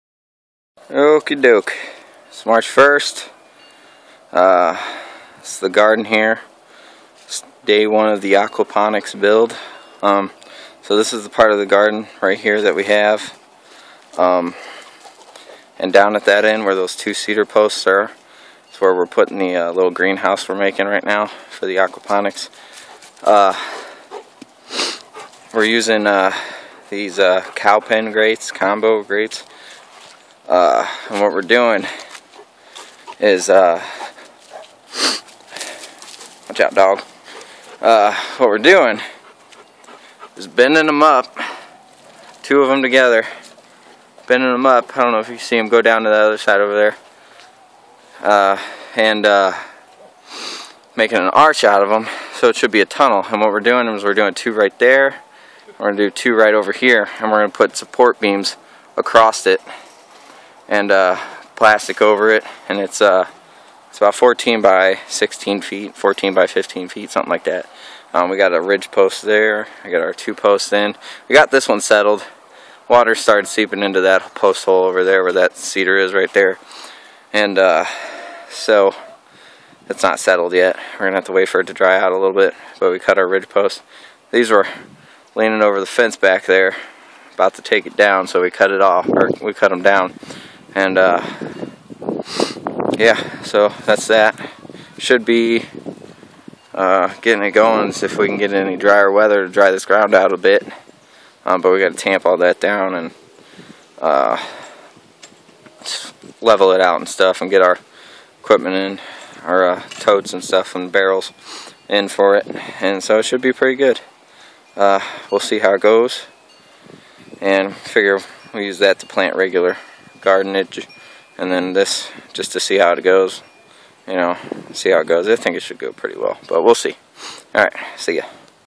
Aquaponics Build Part 1
Starting to build the greenhouse to house the Aquaponics setup. Hoping it goes well. Should be pretty cheap build. We will see.